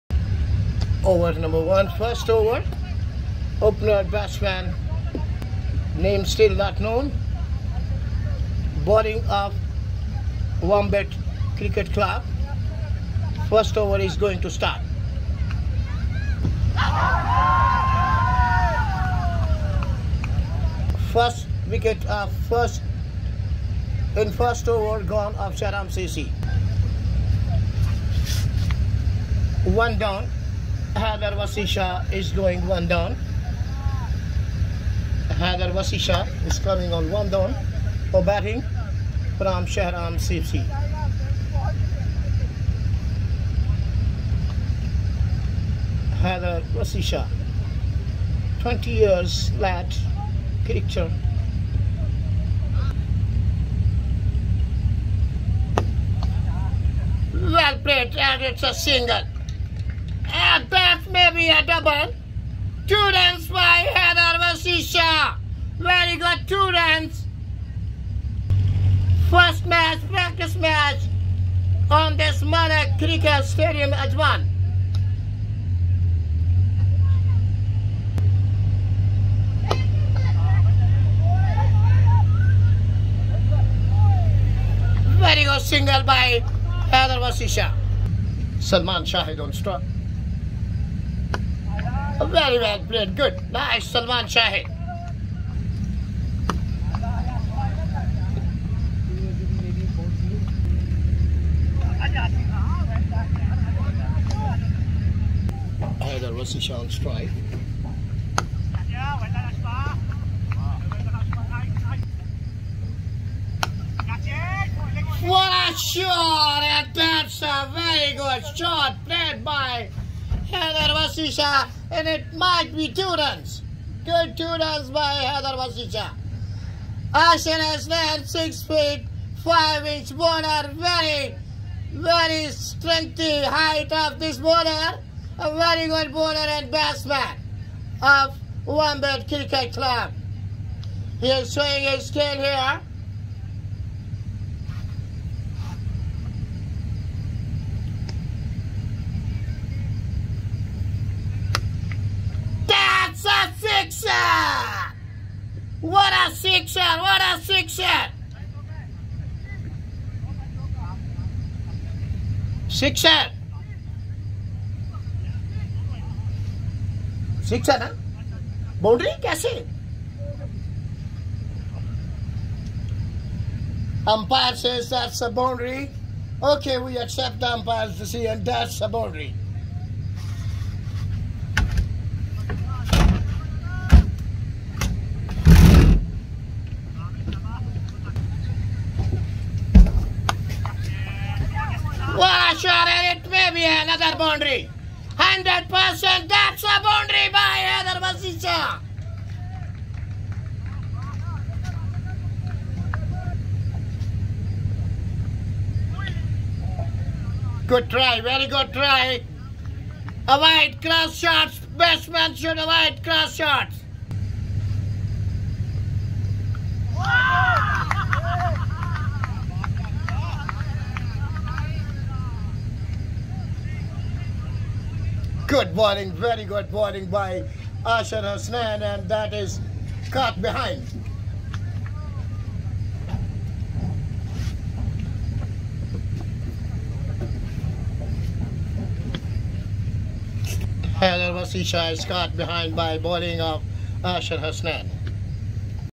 HAIDER WASI SHAH(M=1)803/21 R=13 B=8(SHAHRAM CC PAKISTANvs WOMBAT CC MALEK CRICKET STADIUM AJMAN
16 SEPTEMBER  2021 WASI SHAH's MATCH NO 803/2021 TOTAL RUNS MADE  32,202  50  CENTURIES  228  50's  SINCE  1ST JANUARY  2016.